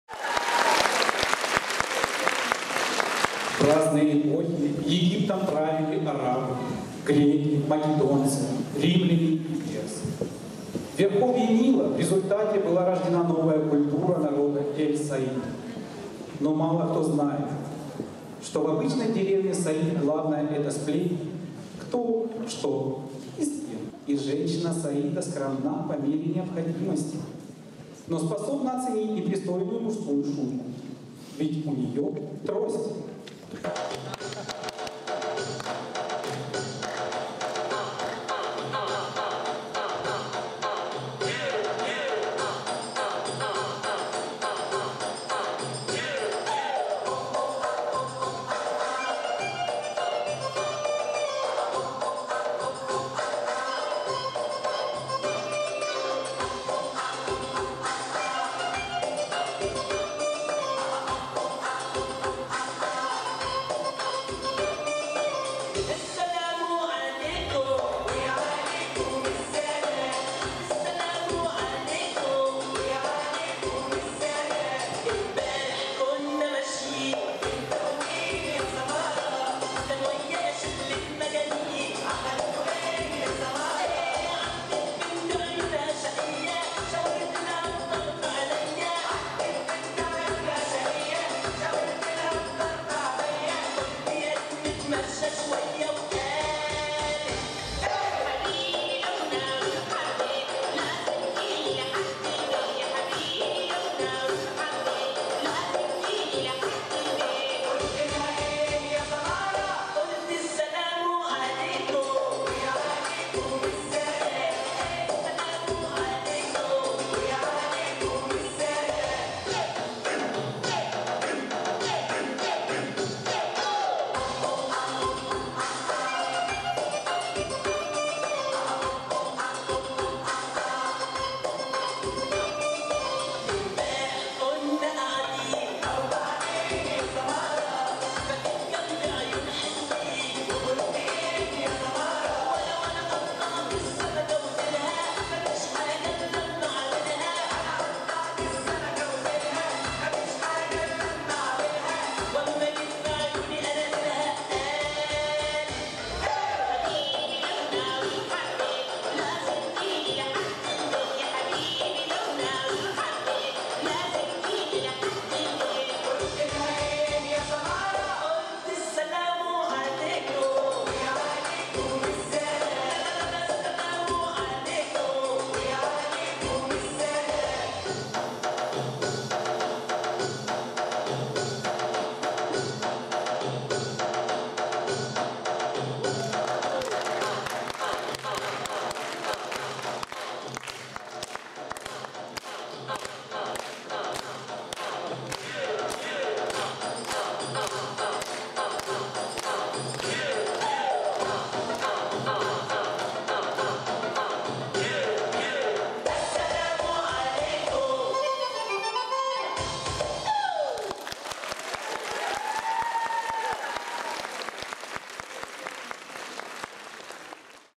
Ukraine Ethno Dance Festival "Живая вода" Саиди. Raks Medan. Чернигов 2014
Живой, горячий восточный танец с тростями на сцене черниговского международного фестиваля этнических танцев "Живая вода" 2014 от наших друзей - школы танцев "Raks medan"

Телефон для справок:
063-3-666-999
Школа танца "Лила"